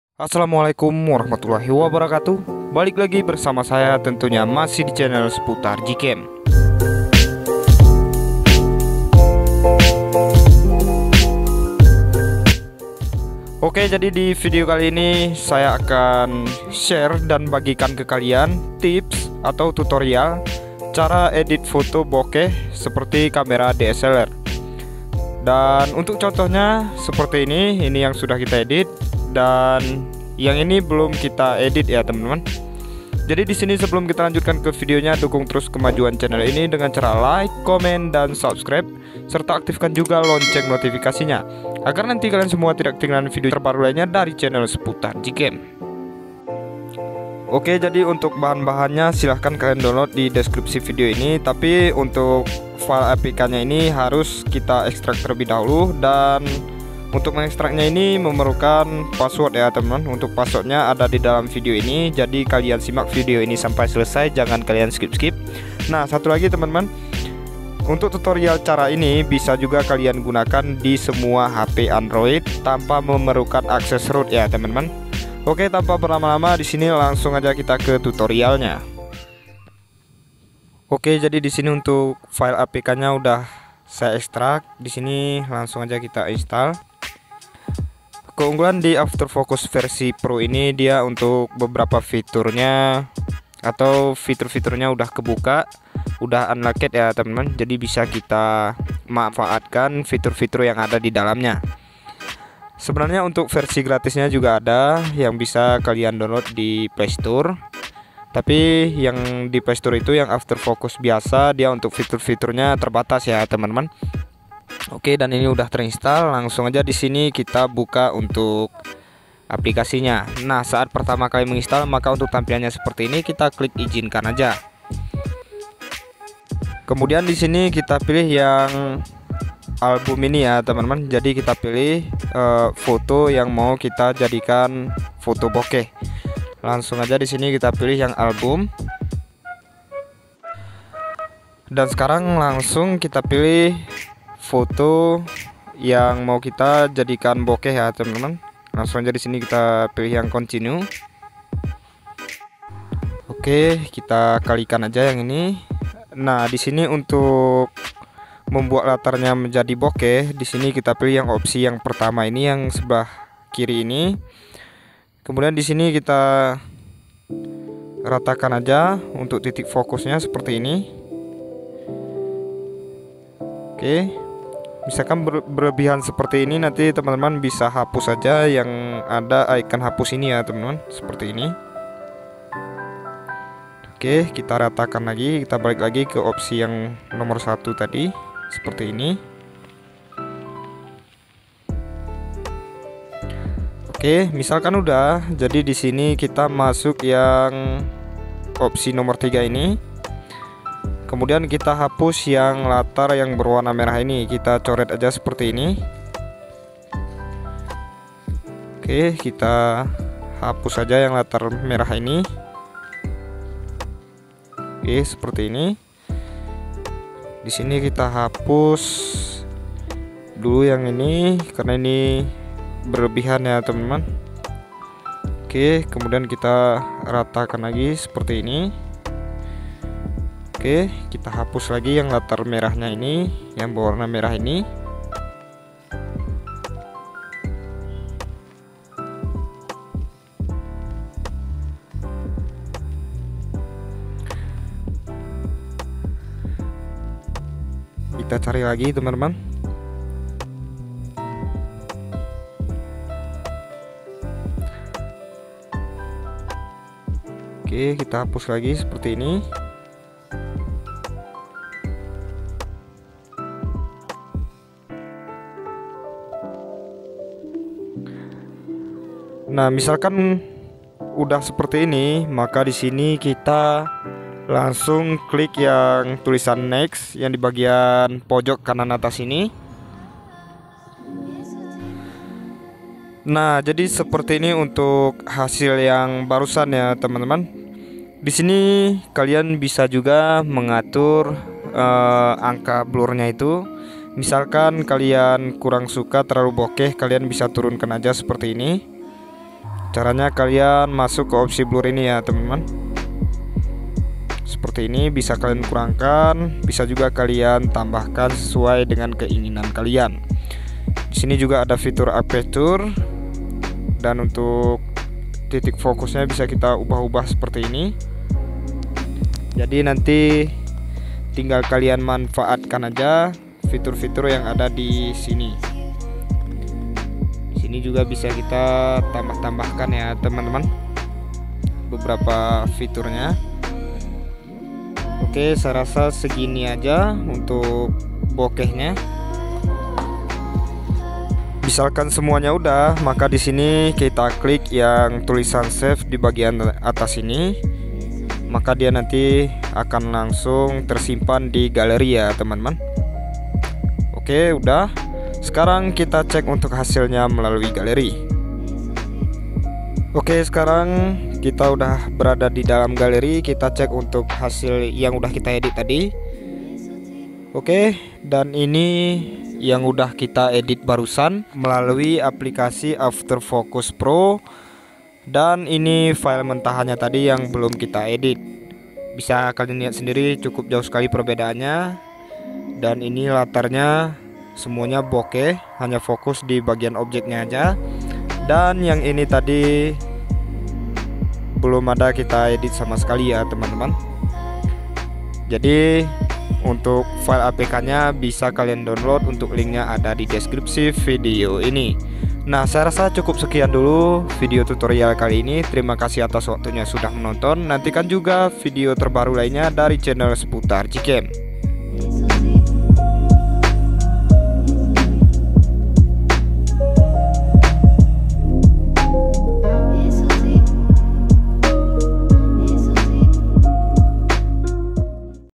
TERBARU🔥CARA EDIT FOTO BLUR / BOKEH SEPERTI DSLR DI ANDROID | BISA DI SEMUA ANDROID TANPA ROOT
( Password Config Ada di bagian tengah atas Video muncul hanya 4 detik )

Simpan File Config nya di Folder Gcam/Config7